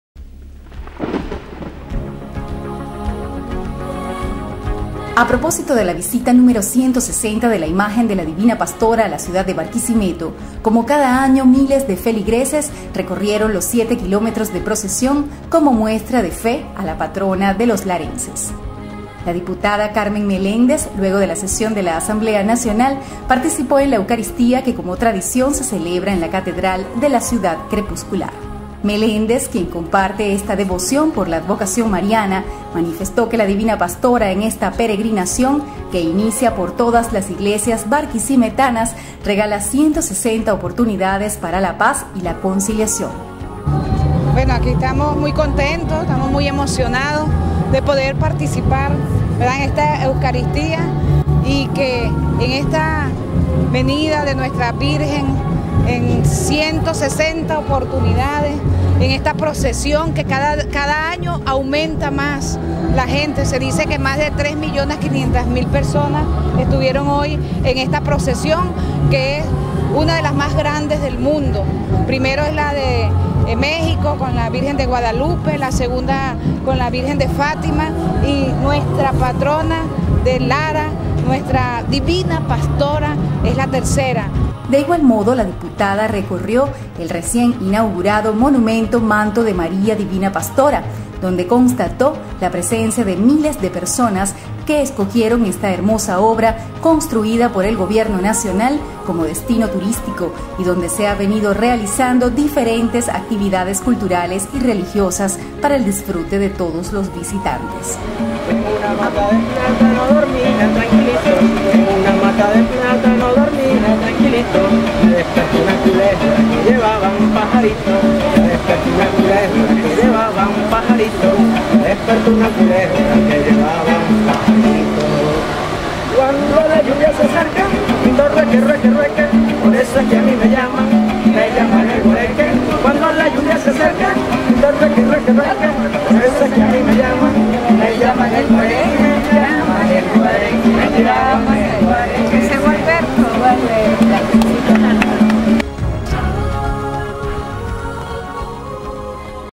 Divina Pastora nos regala 160 oportunidades para la paz y la conciliación de todos los venezolanos
La diputada Carmen Meléndez, luego de la sesión de la Asamblea Nacional, participó en la eucaristía que como tradición se celebra en la Catedral de la ciudad crepuscular